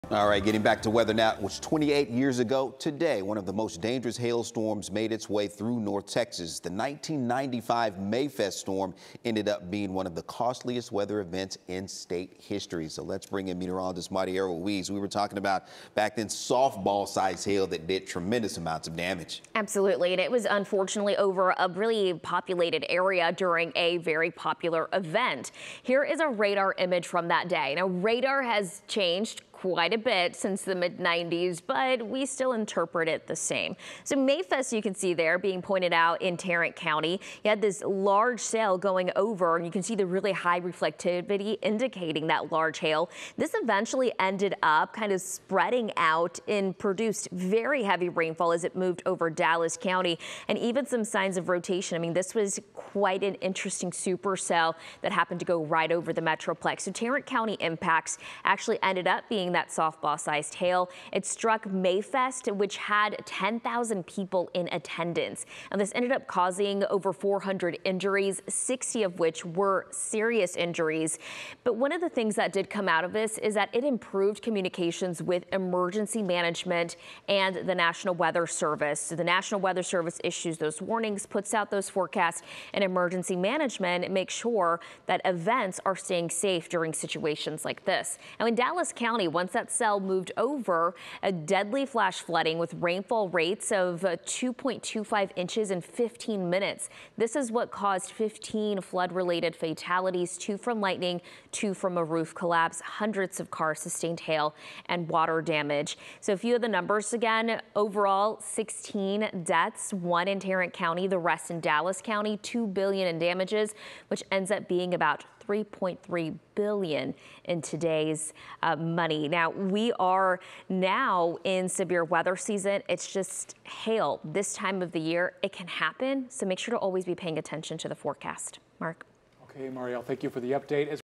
DFW weather: Looking back on the dangerous 'Mayfest' storm
The Mayfest storm ended up as one of the costliest storms in Texas history.